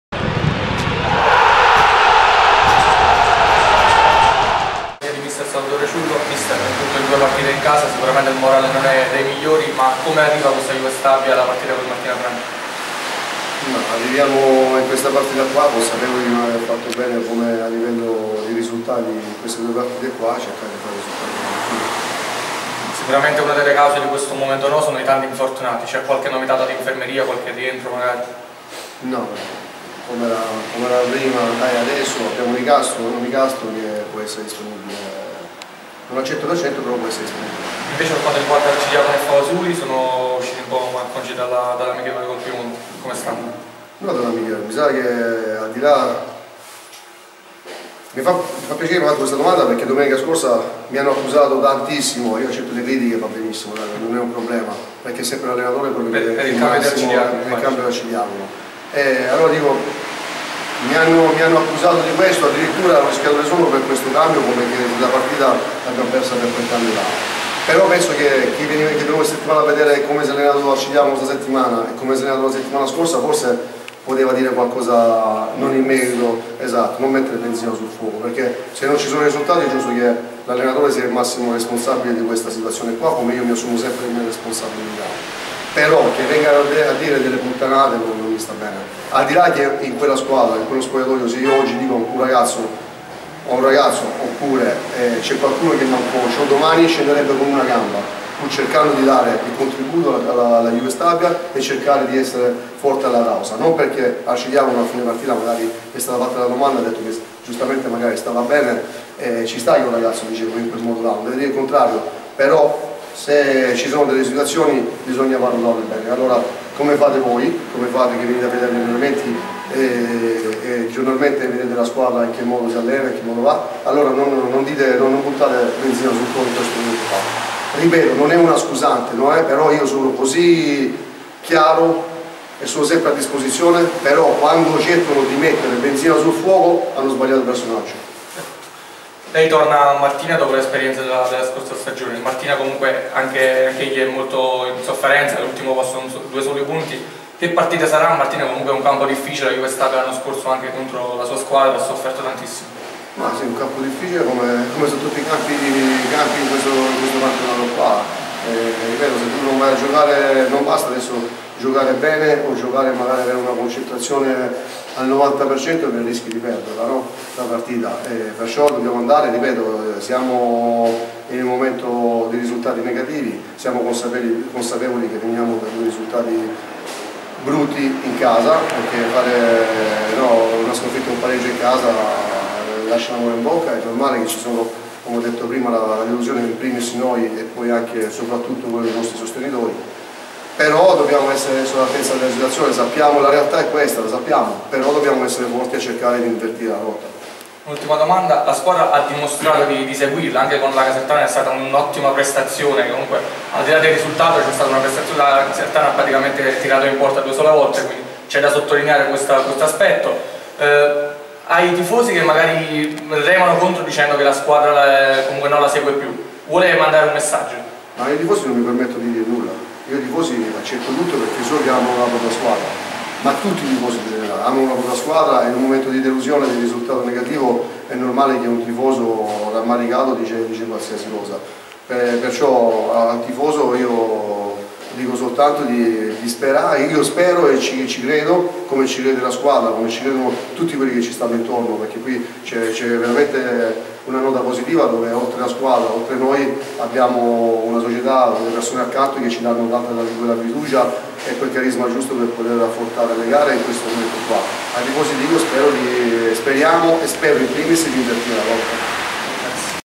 Mister Ciullo | 10-10-2015
Le dichiarazioni di mister Salvatore Ciullo alla vigilia di Martina Franca-Juve Stabia, gara valevole per la 6a giornata del Girone C della Lega Pro Divisione Unica.